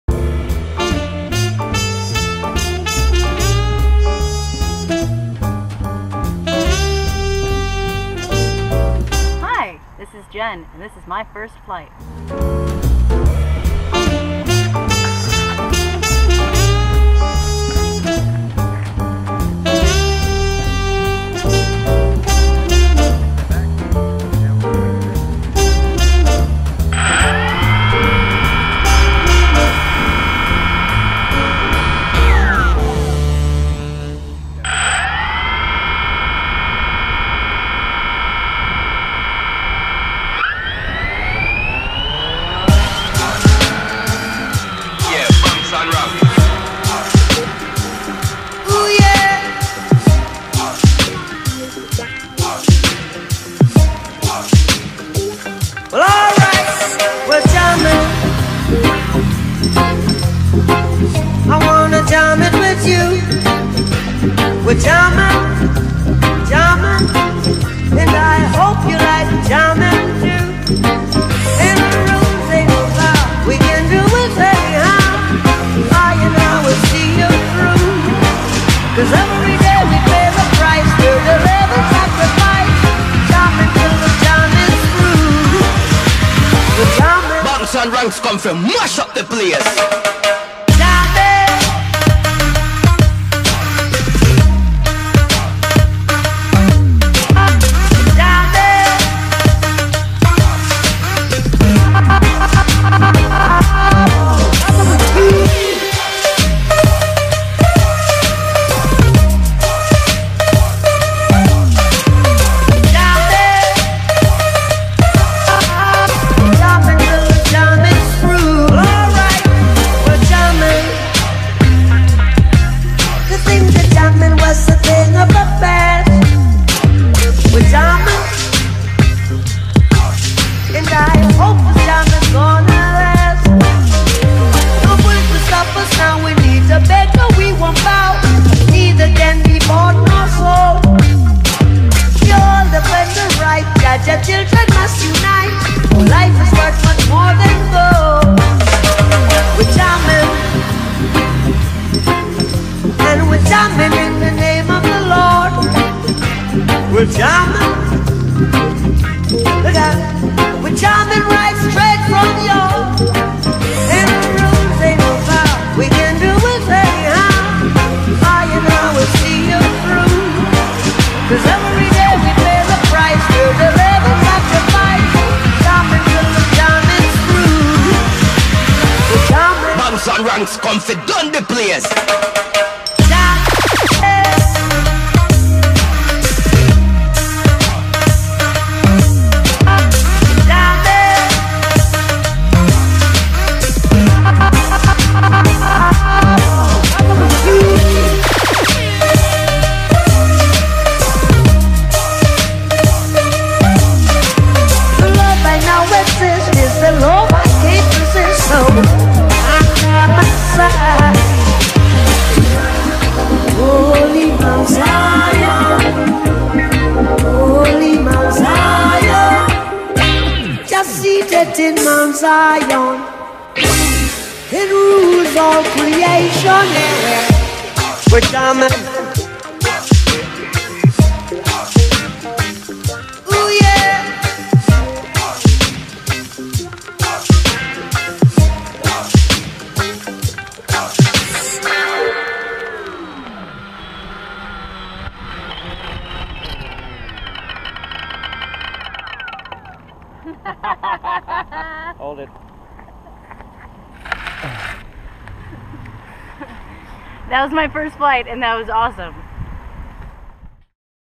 JEN'S FIRST FLIGHT
This video is about Jen flying a UAV for the first time in light wind over KIrkwood.